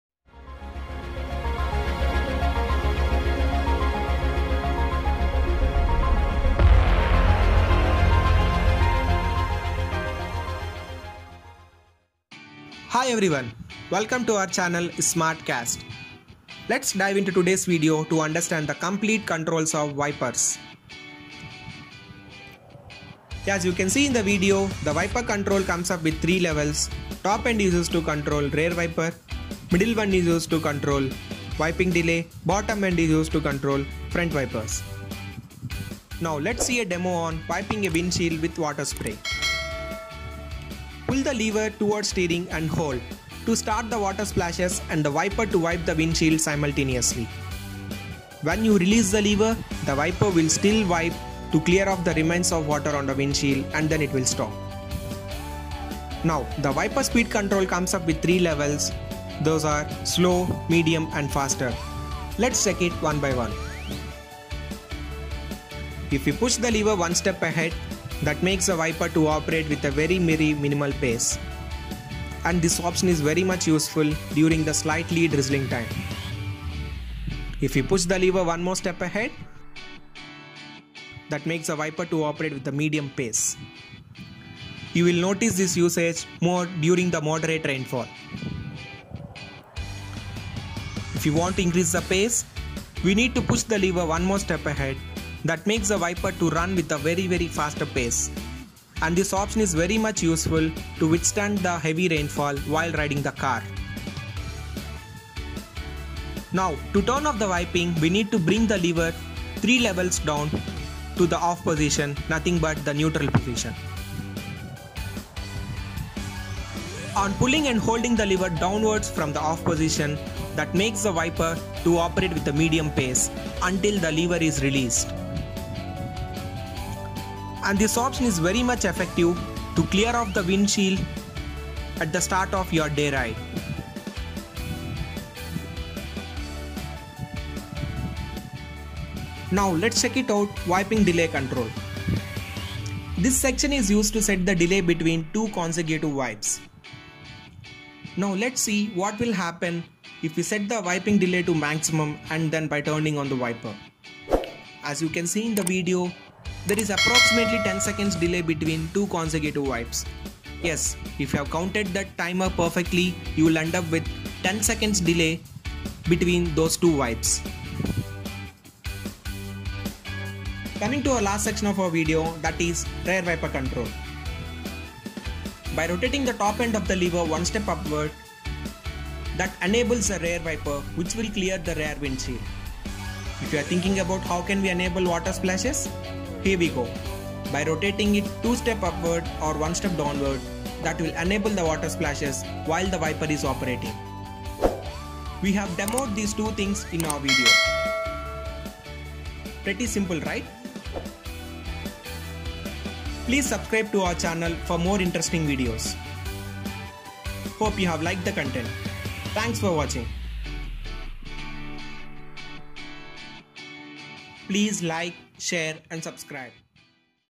WIPER CONTROLS | FRONT & REAR WIPERS | WIPING WITH WATER | SPEED & DELAY CONTROLS | TATA CARS
Learn How to control Front & Rear wipers at different speeds & wiping delays. Know how to use water splashing & wiping. Explained in TATA Tiago. These steps are common for almost many cars.